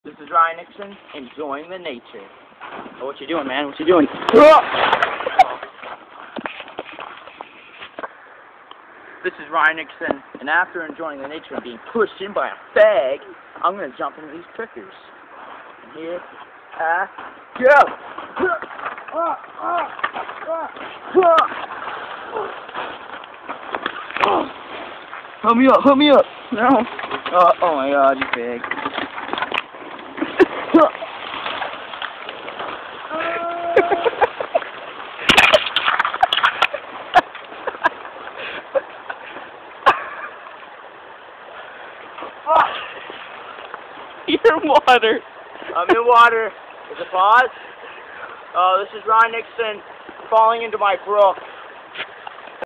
Bush jumping
Ryan jumping in bushes